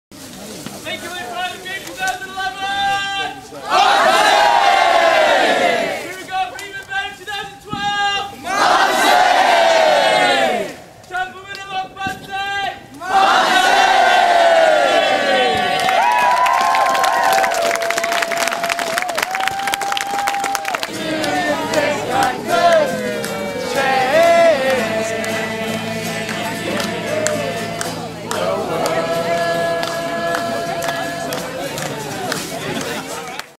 Start New Year at Camp Innabah
The staff and participants of the Shehaqua Winter Retreat at Camp Innabah start the New Year 2012 together around a bondfire